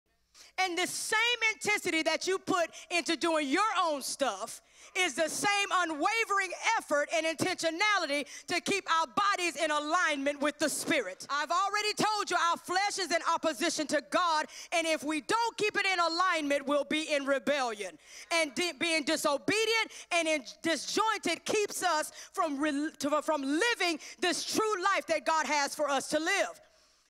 Pastor LaTonyea Leonard - If we don’t keep it in alignment we’ll be in rebellion
“How You Livin?”
Pastor LaTonyea Leonard
The Just Shall Live by Faith-Part 5

Join us in the house Sunday at 11am
This video is the property of New Season.
Broadcasts do not generate funds.